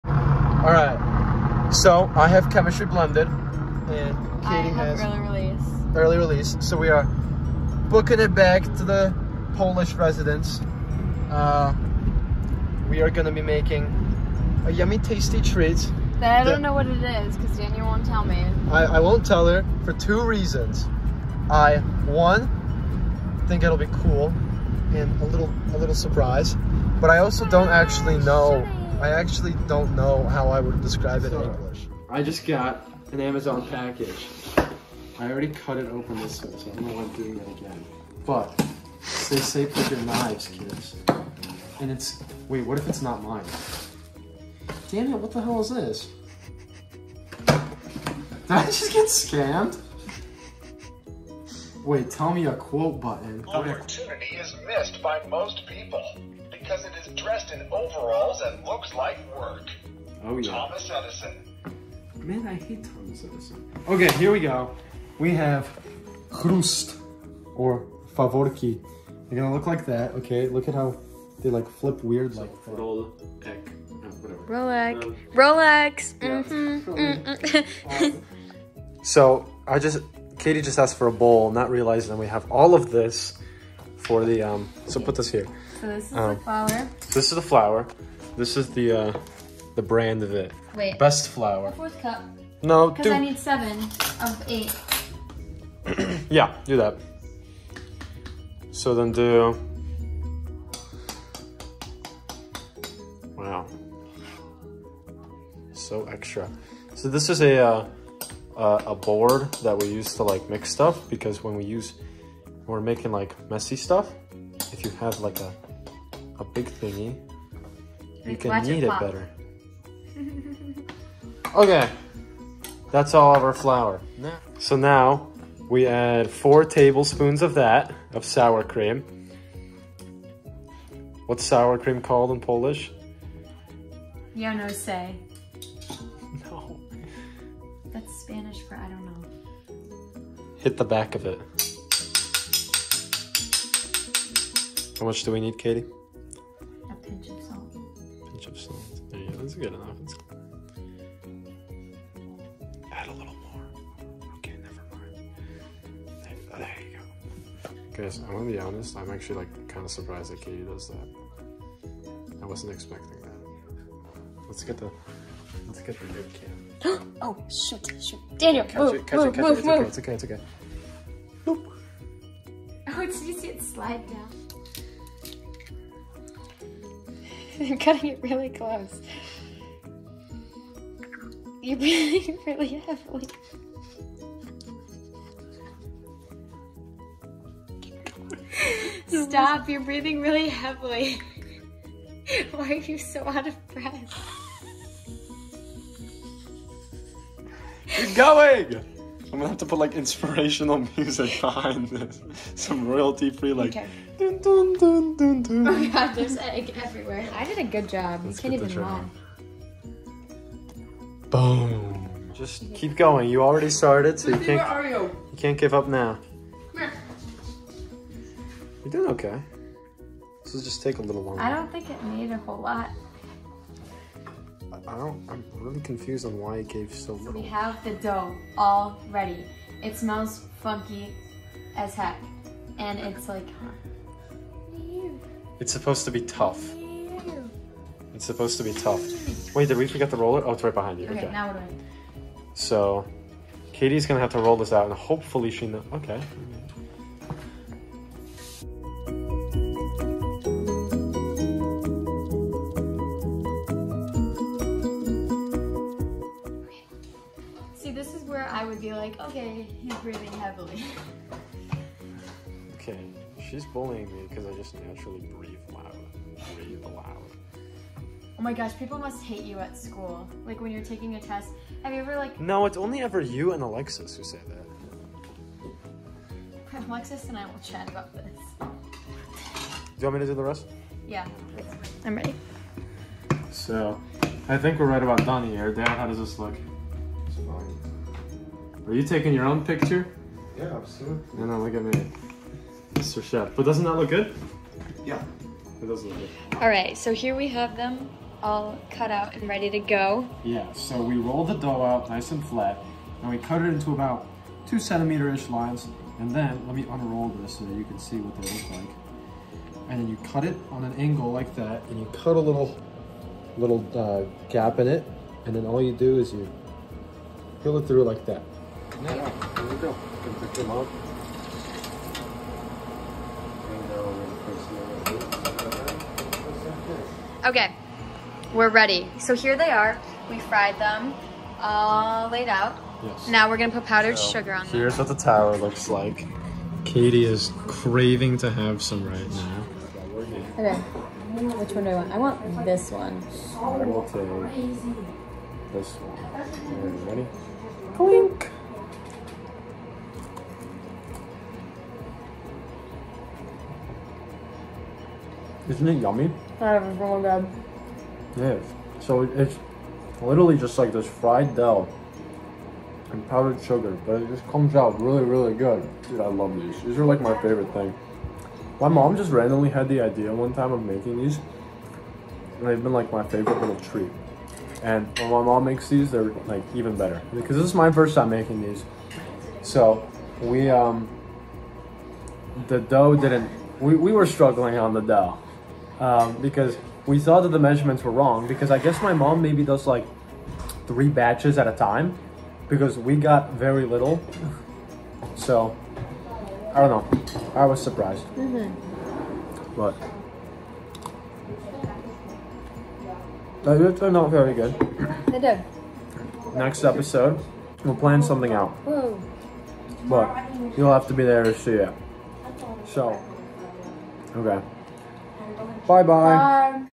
Deli 4 Us: Episode 2
Join Daniel Przybylko and Katie Walsh as the cook and review Faworki.